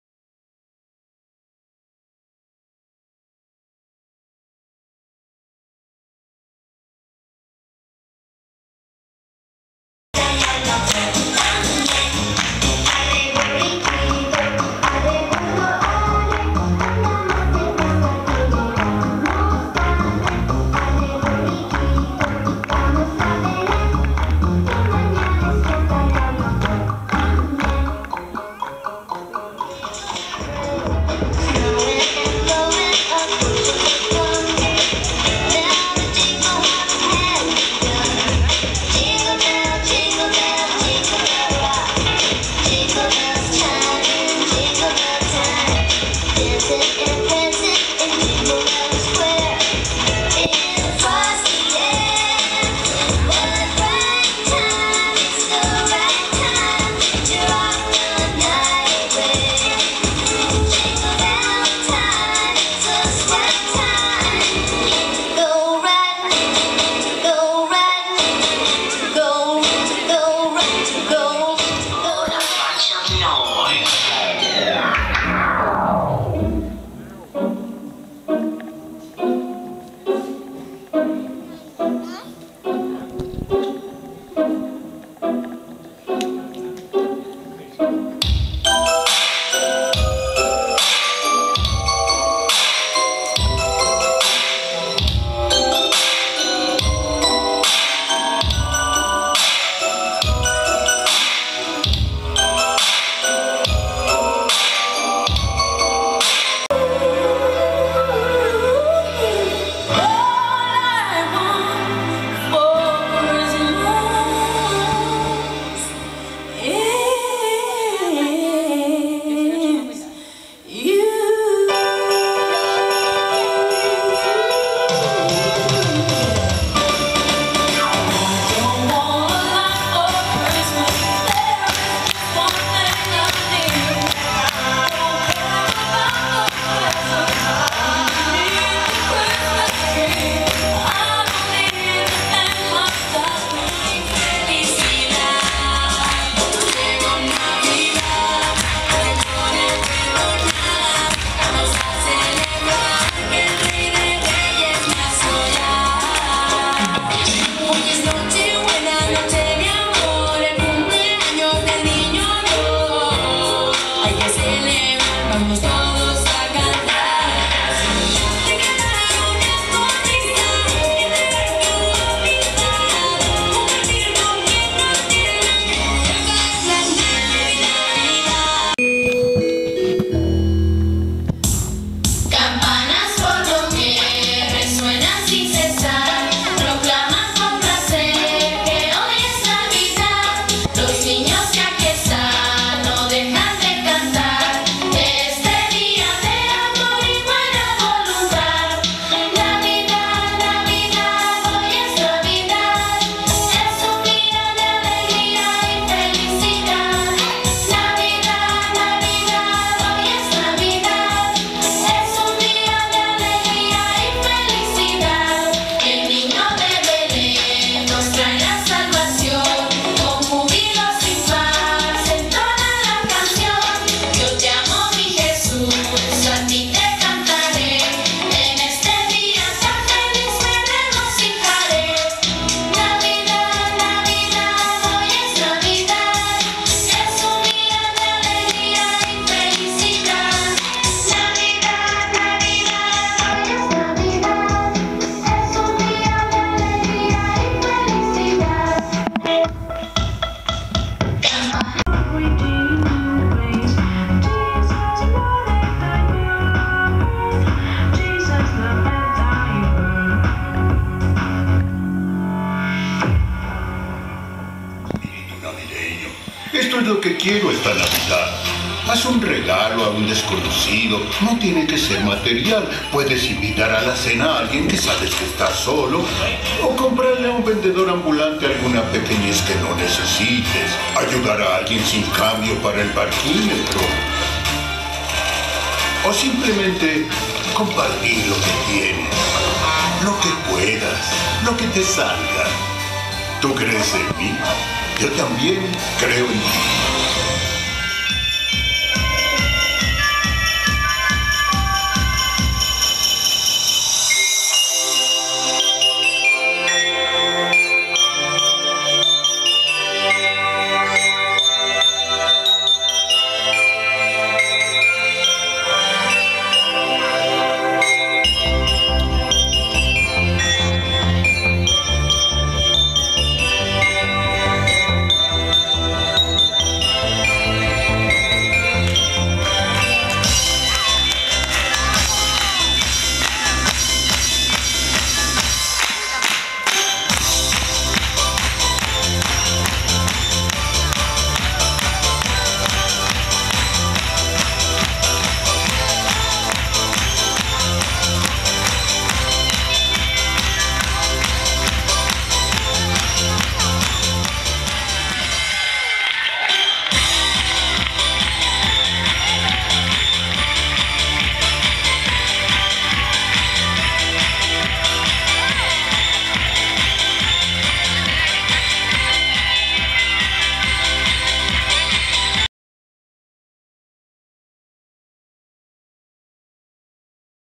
NADAL SOLIDARI. Per un somriure
Popurri-resum de les actuacions al Teatre de Capdepera. La recaptació és a benefici d'APROSCOM